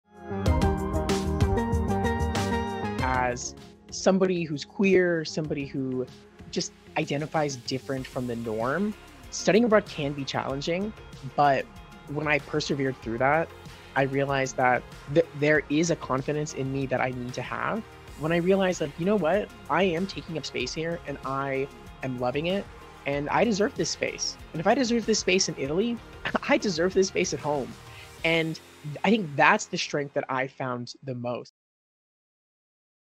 Coming Soon: Building confidence, community, and an appetite
It helps to be a part of a deep-rooted Italian community if you’re looking to be taught how to cook pasta the “right” way. Accepting new ways of doing things can also help you build the confidence to teach others about your own values as well. University of Nevada Las Vegas graduate Ryder Hankins will walk us through the lasting impact of making good food and finding a friendly community as a queer person during his time in both the USAC Viterbo and USAC Torino programs. In this episode, we will also feature a segment of “You Belong,” a video presentation by USAC Chiang Mai alumnus Ami King, about his experience finding community as a queer person abroad.